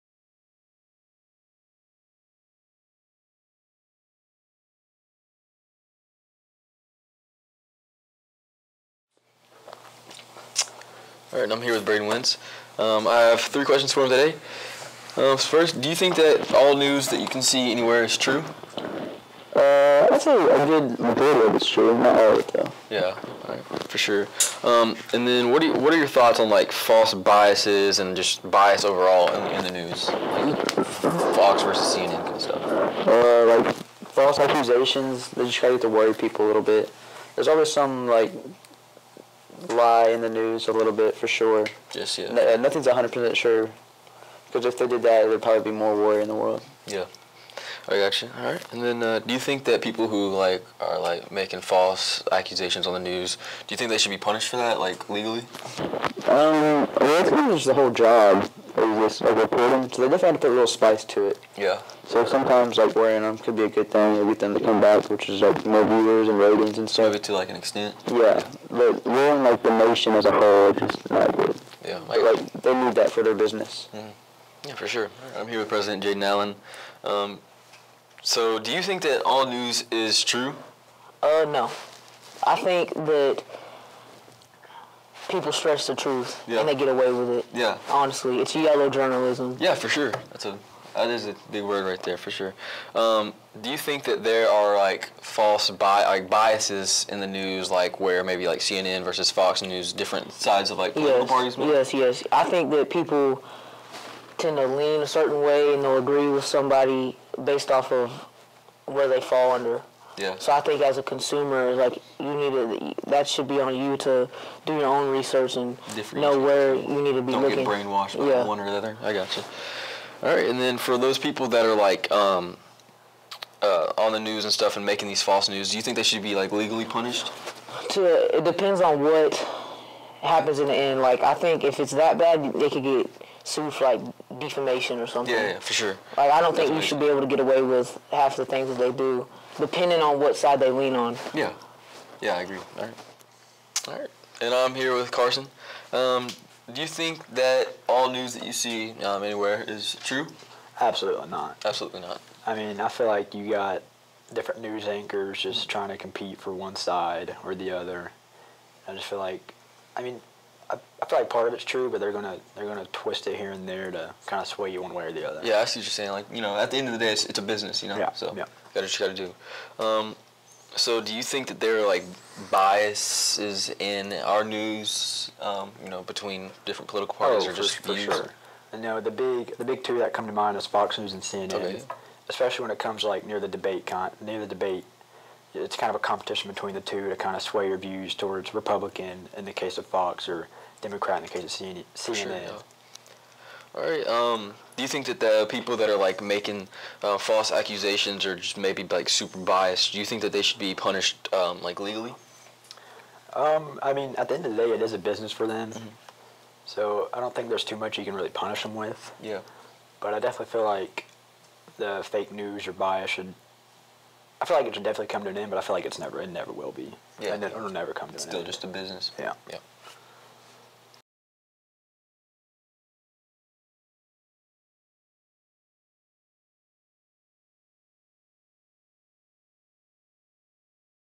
Hargrove Ethics in Journalism
Interviewing my classmates about their thoughts on bias media